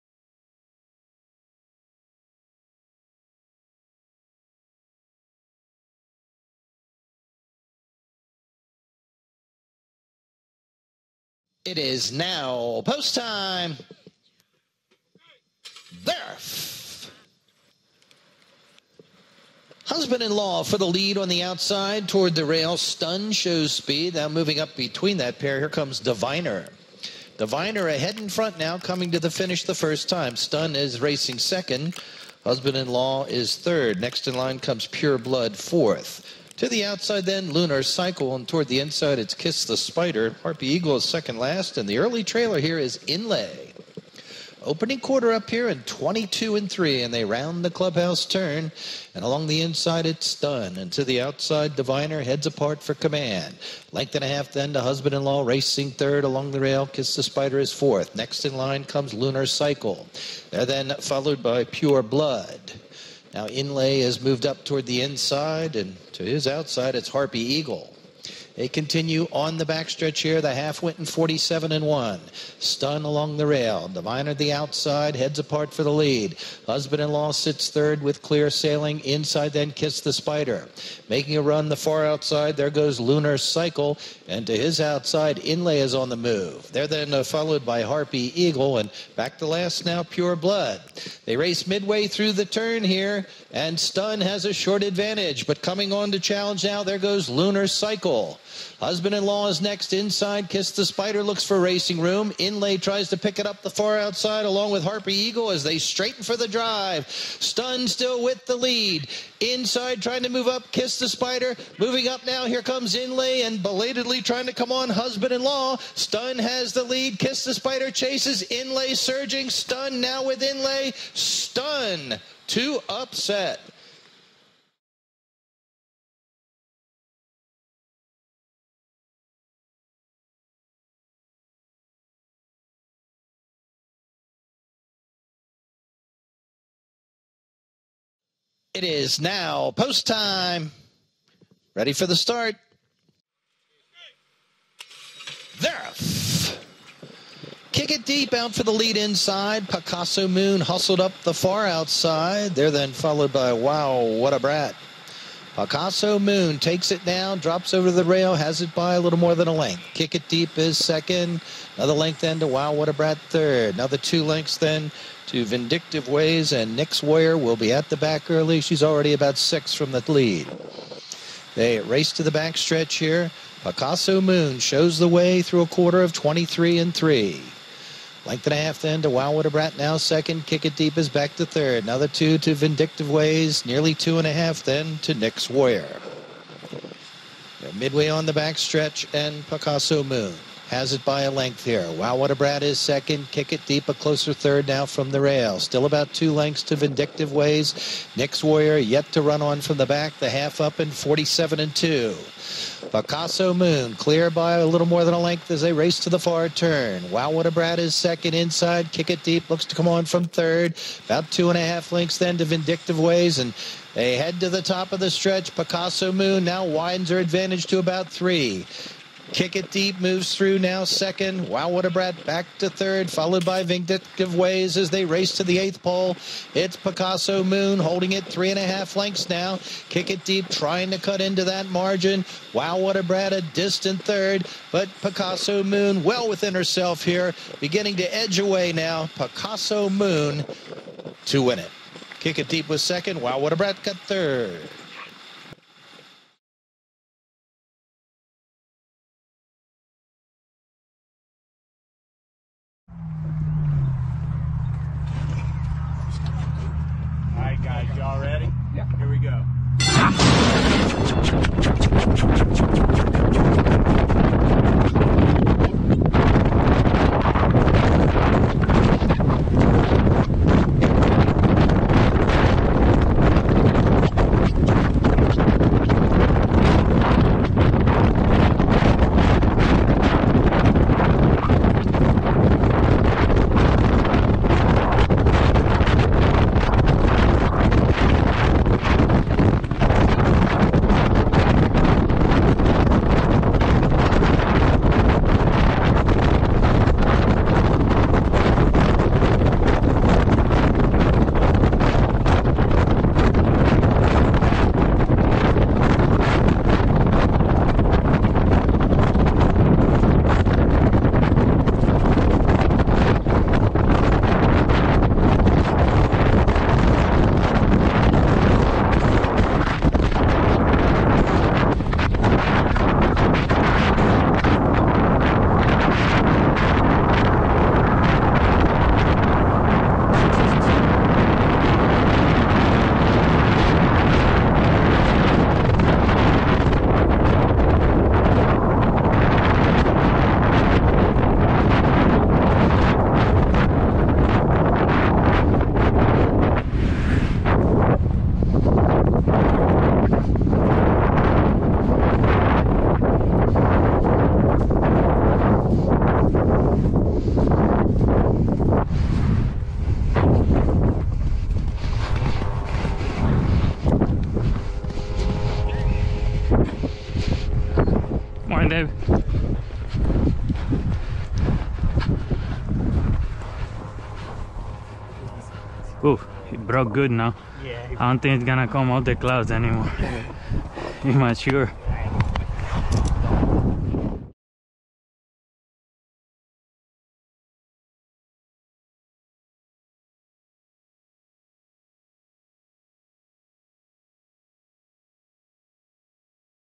Take a Ride on a Thoroughbred Racehorse!
Experience what it is like to ride a Thoroughbred during morning training, and in races on two different surfaces, turf and dirt!

A special thank you to Godolphin and the Pennsylvania Horse Racing Association for supplying the footage!